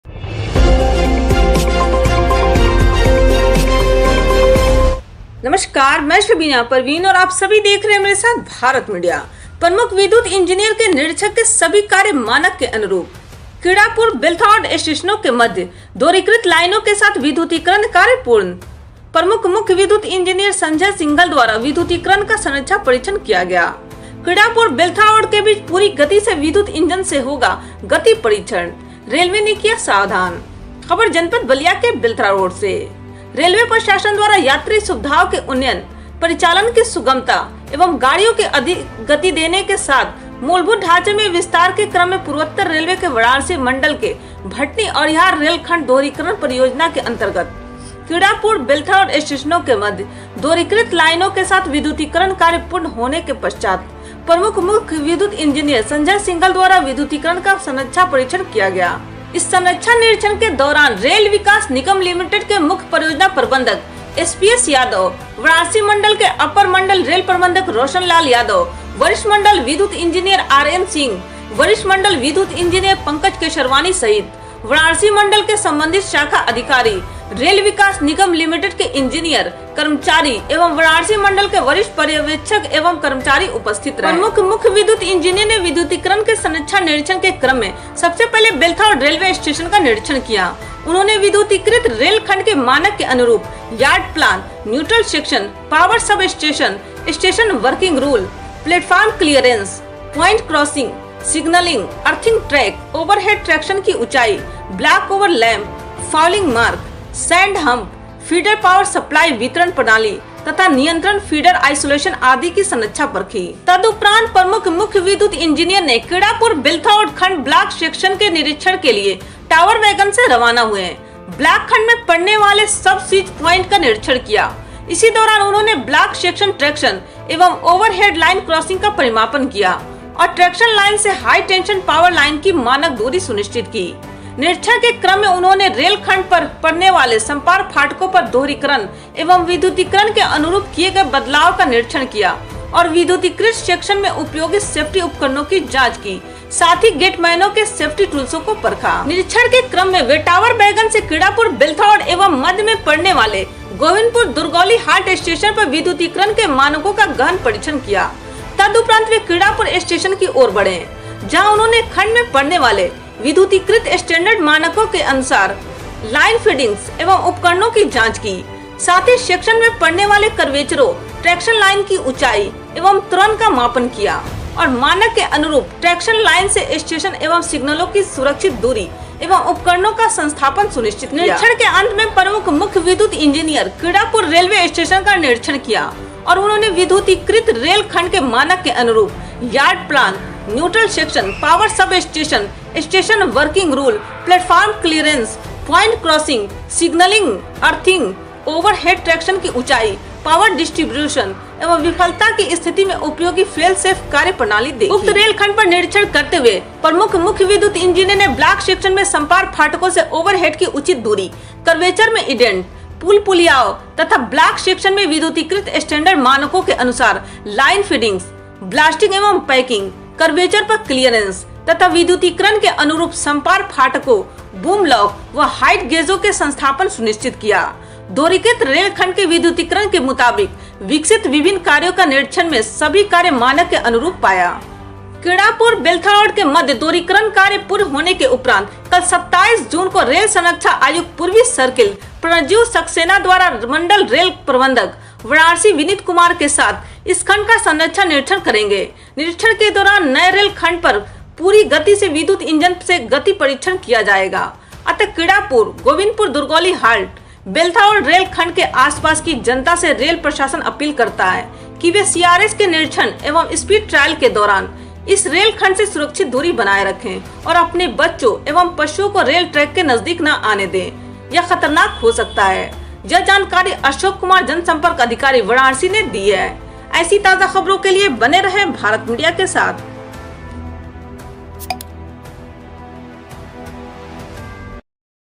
कीड़िहरापुर-बेल्थरा रोड के बीच पूरी गति से विद्युत इंजन से होगा गति परीक्षण ,रेलवे ने किया सावधान..!
प्रमुख विद्युत इंजीनियर के निरीक्षण में सभी कार्य मानक के अनुरूप

कीड़िहरापुर-बेलथरा रोड स्टेशनों के मध्य  दोहरिकृत लाइनों के साथ विद्युतीकरण कार्य पूर्ण

 प्रमुख मुख्य विद्युत इंजीनियर  संजय सिंघल द्वारा विद्युतीकरण का संरक्षा परीक्षण किया गया